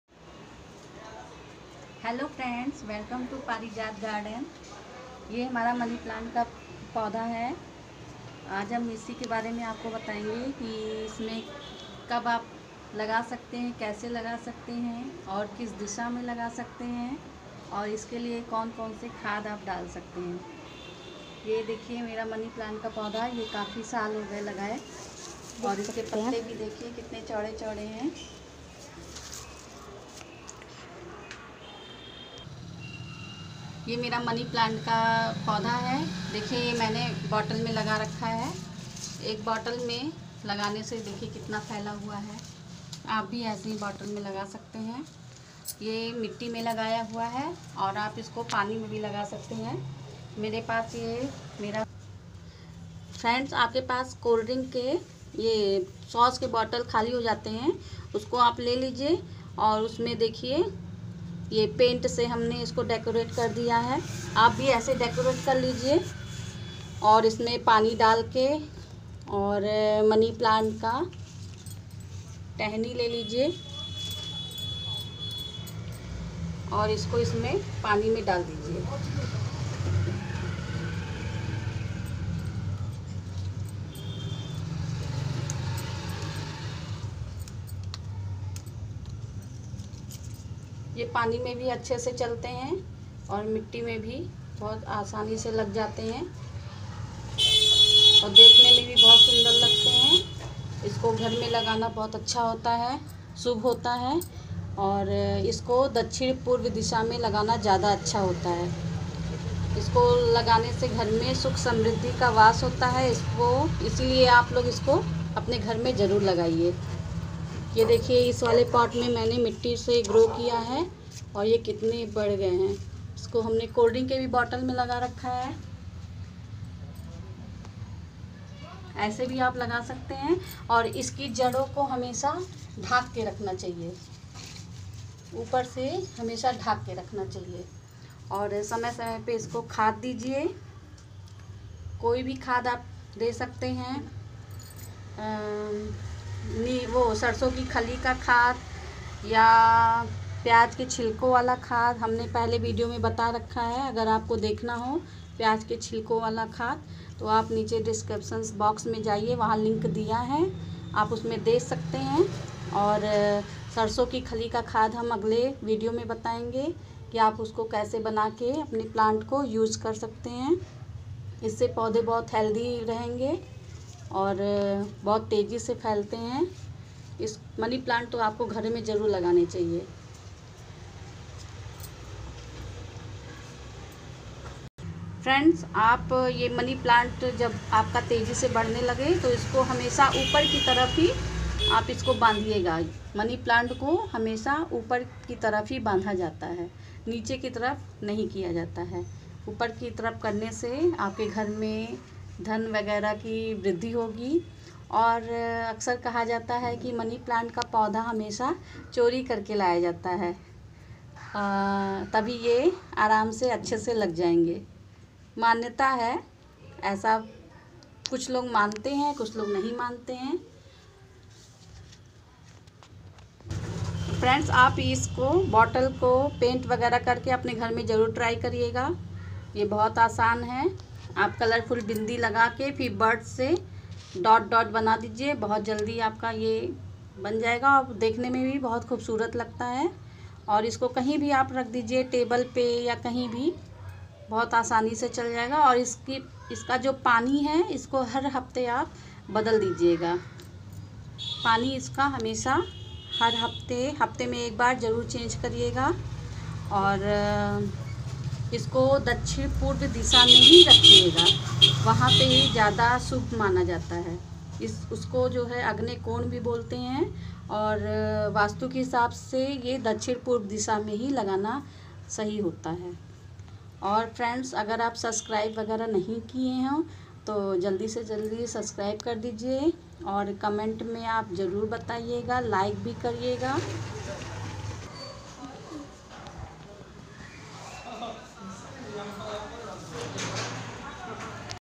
Money plant Sukh Samriddhi ka Pratik
Mani plant kis Disha Mein lagaen or kya khad dale